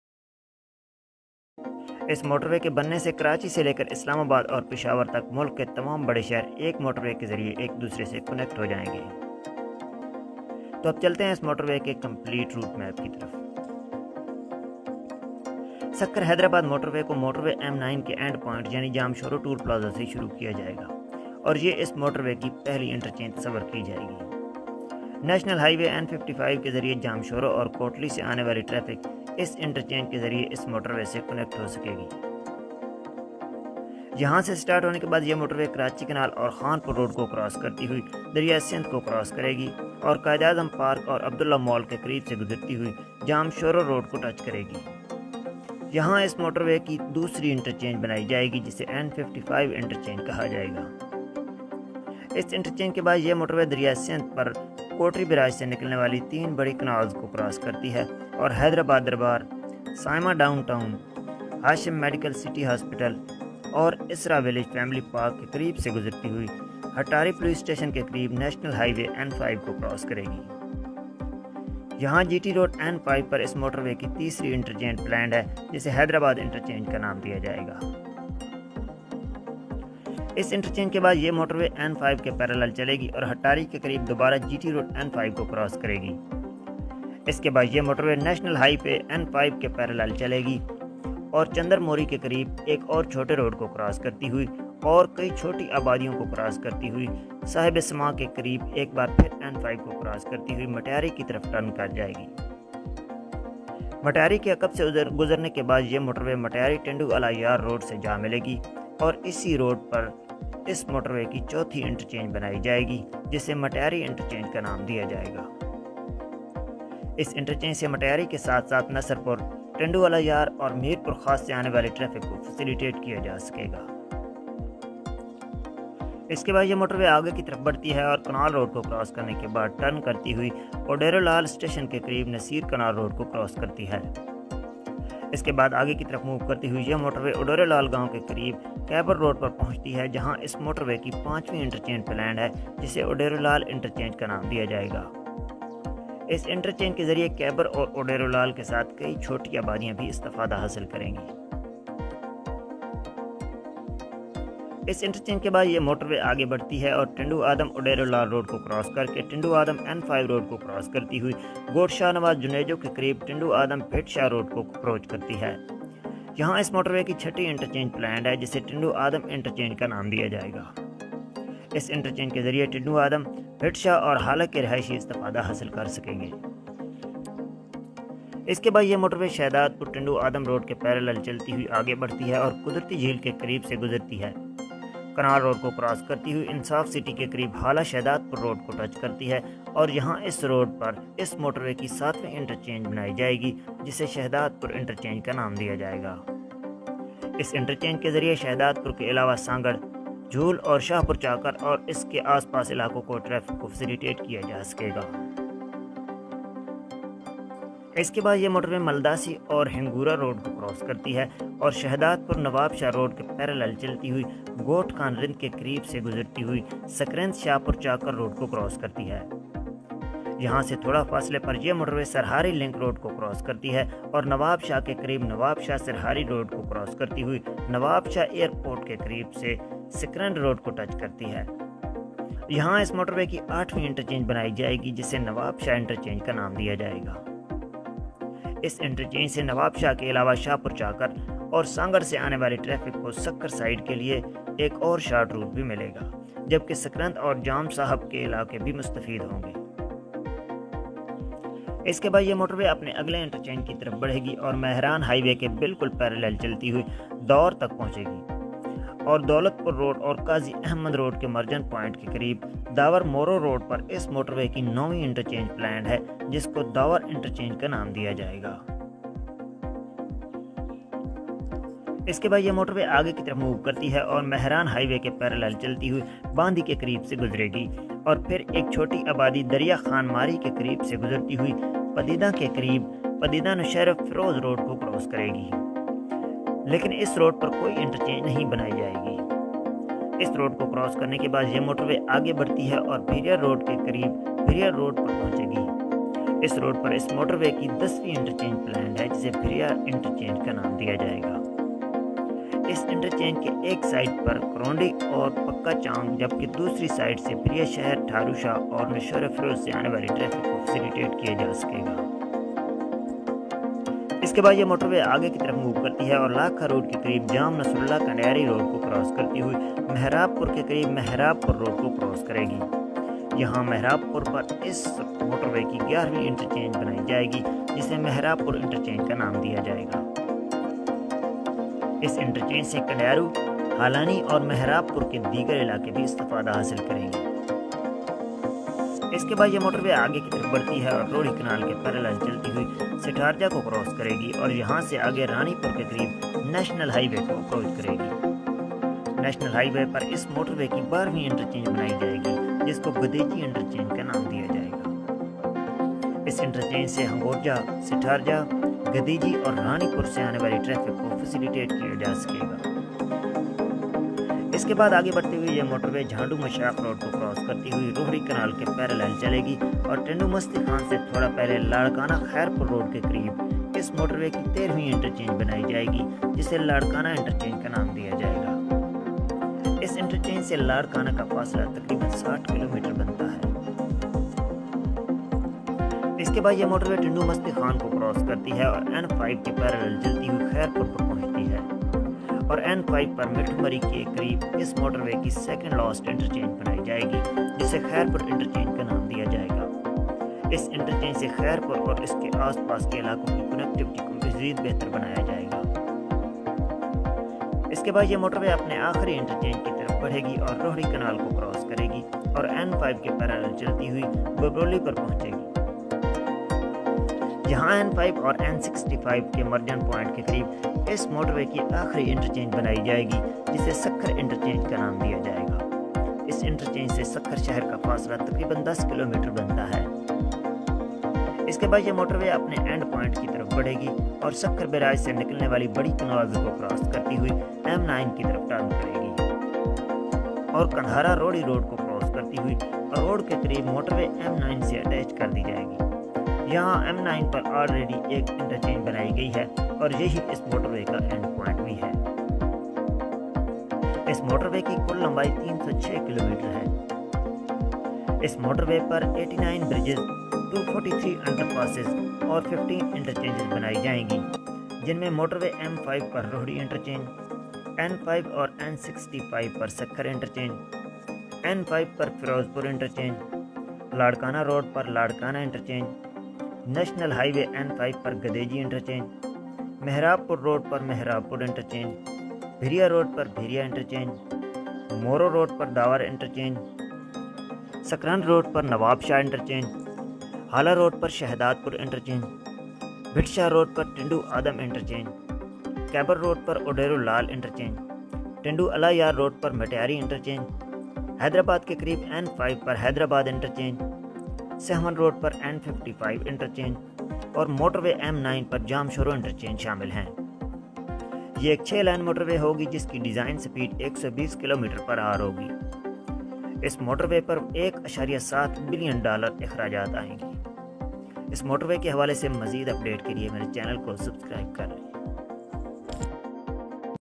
Sukkur to Hyderabad motorway 👈🏻👈🏻
لازمي ڏسڻ جھڙي وڊيو ـ موٽر وي حيدرآباد  سکر جو نيو نقشو ـ جنهن م ھر تفصيل آهي.